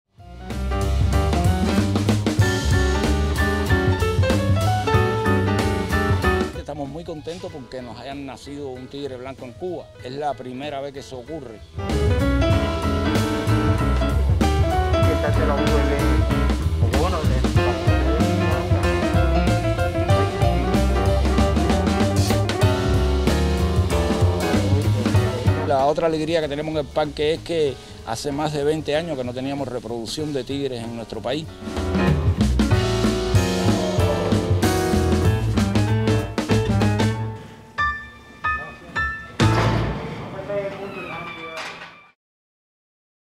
Primeiro tigre branco cubano | AFP
Uma alegria dupla para os funcionários do Zoológico Nacional de Cuba: o nascimento de quatro tigres pela primeira vez em mais de 20 anos e, entre eles, a presença sem precedentes de um filhote branco, uma fêmea chamada Yanek.